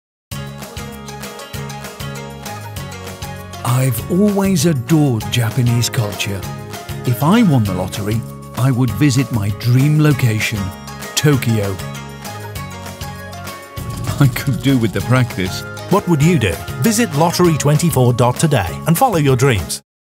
lottery24.today TV Advert: Travel
Square Elephant Productions produced a series of 20-second adverts for lottery24.today, with this advert focusing on an ageing dreamer who wishes to travel far East and bask in the culture of Japan.

Playing the lottery with lottery24.today is a fun way of helping to raise funds for a great cause, and while doing so, still gives you the chance to win! It’s a win-win situation!